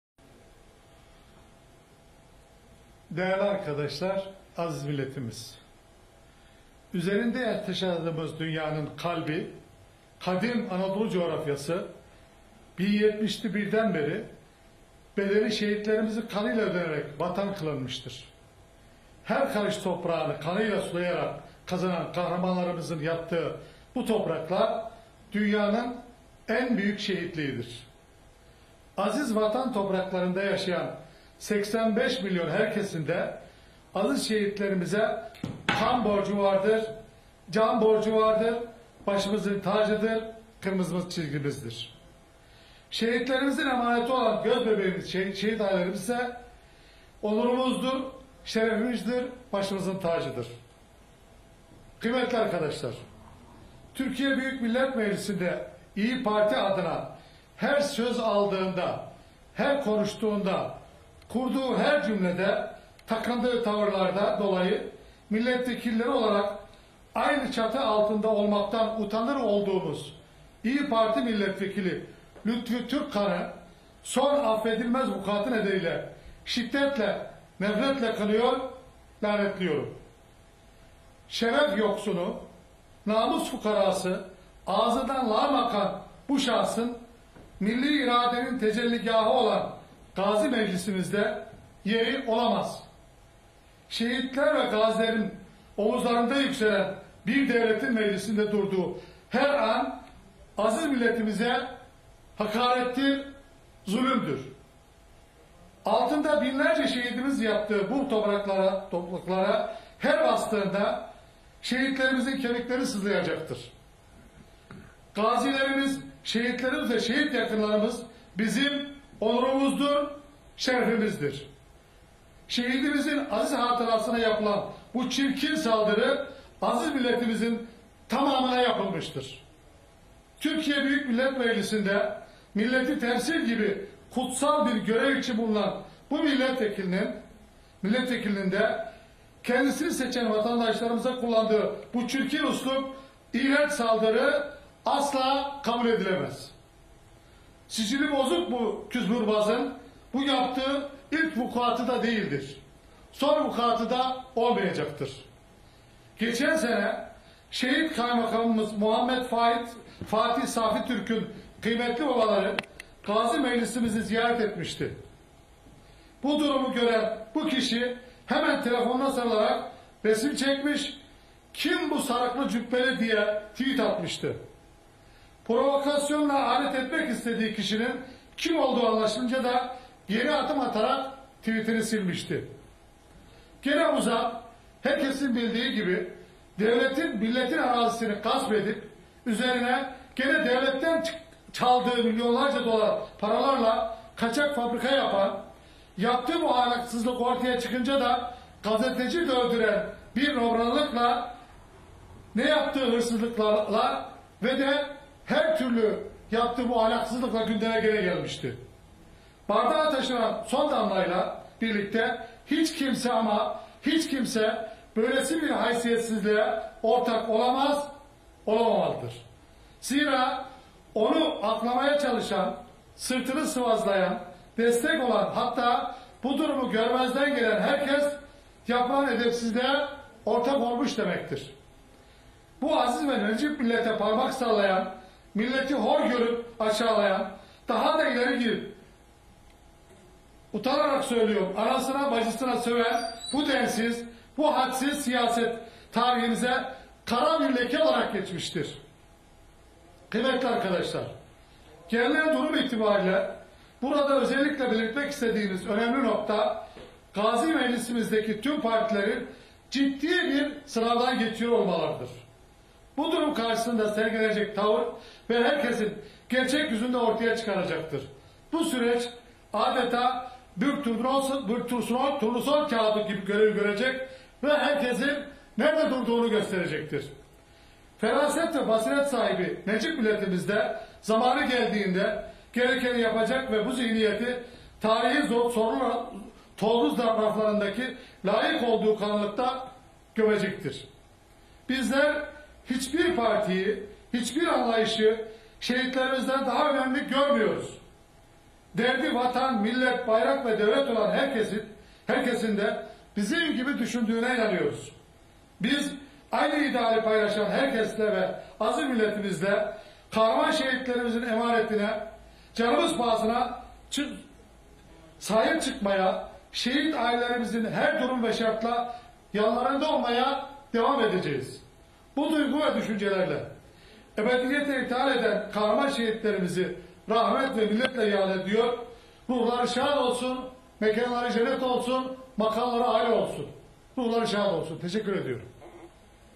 TÜRKKAN'A KAVUNCU TEPKİSİ
AK Parti Çorum Milletvekili Erol Kavunçu, şehit yakınına küfür etmesiyle gündem olan İyi Parti Kocaeli Milletvekili Lütfi Türkkan’a sert tepki gösterdi.